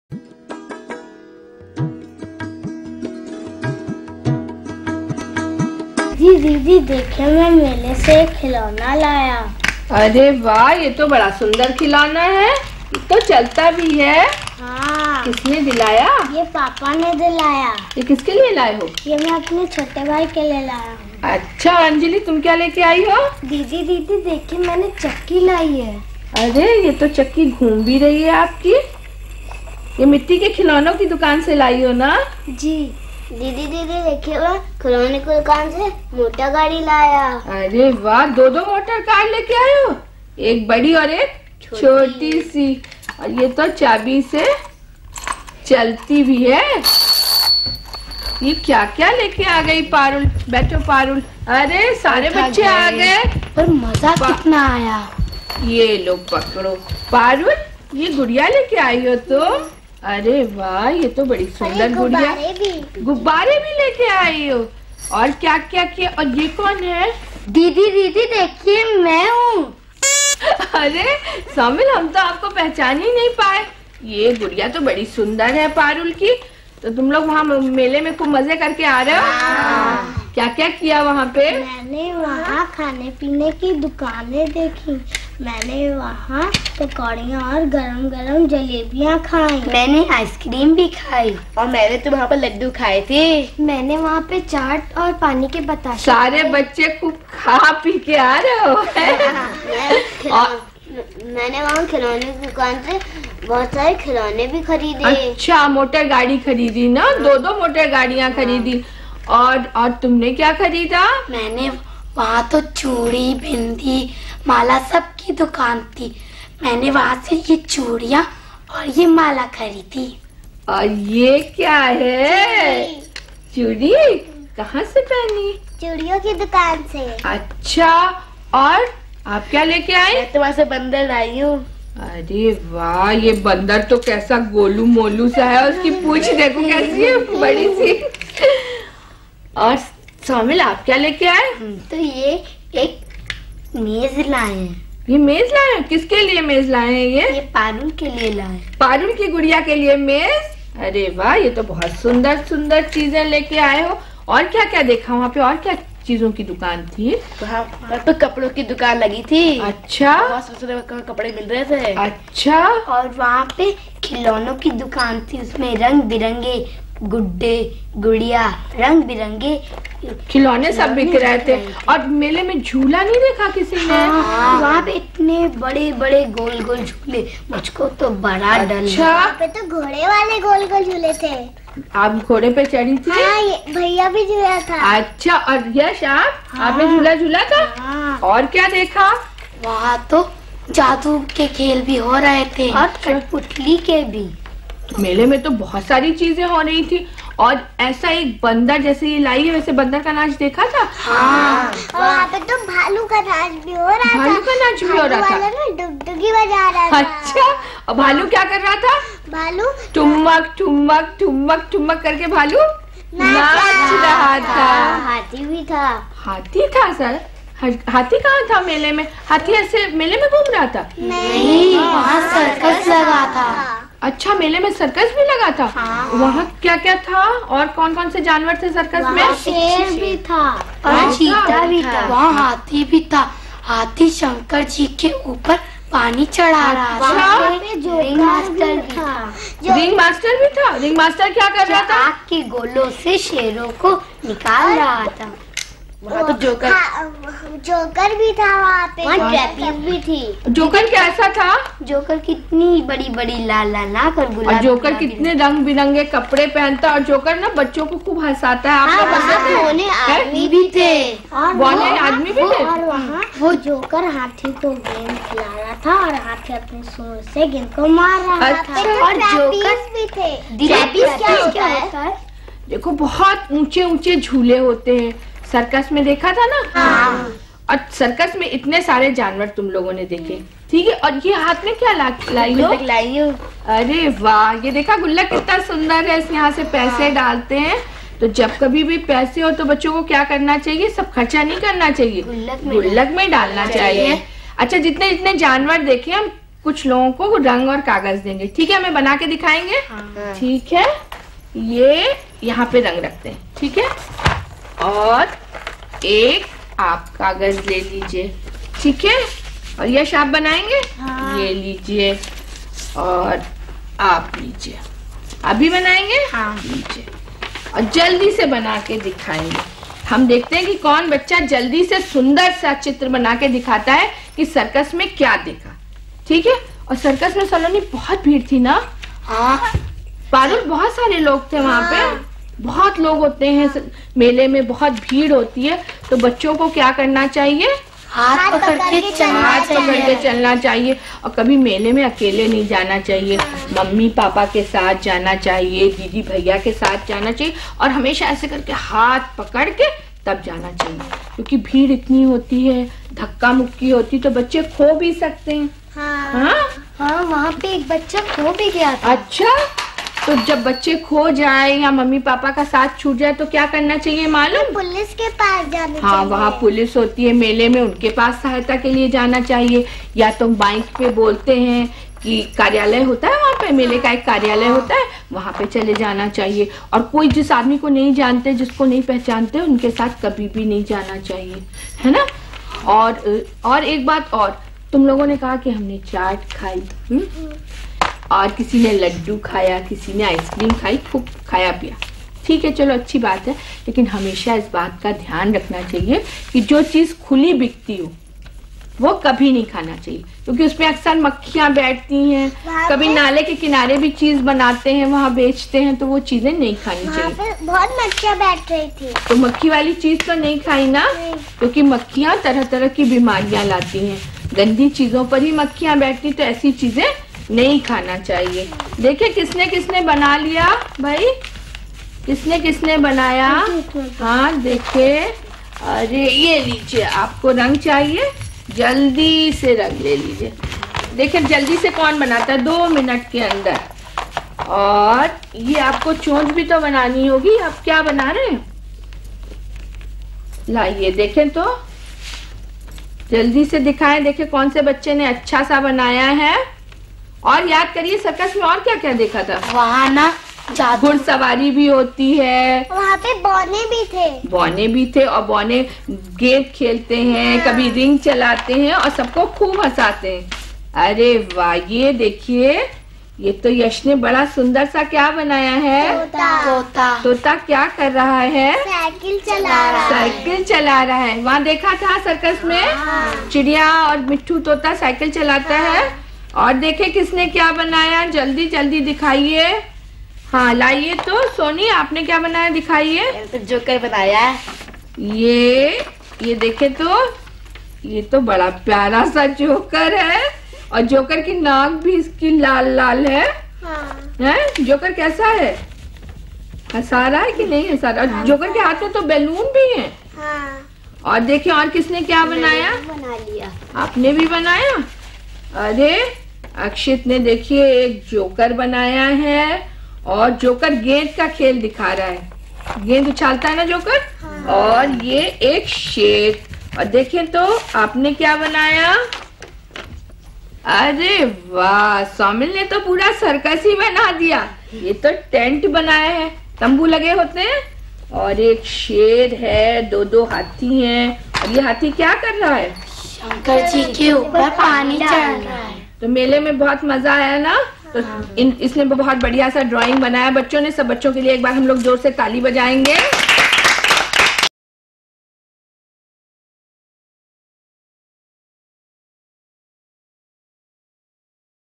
Mele Mein Jhule Ka Khela / मेले में झूले का खेला
इस कार्यक्रम में अध्यापिक बच्चों का मेले में जाने के अनुभवों को पूछती है और उनके शब्दकोश में वृद्धि करने का प्रयास करती है।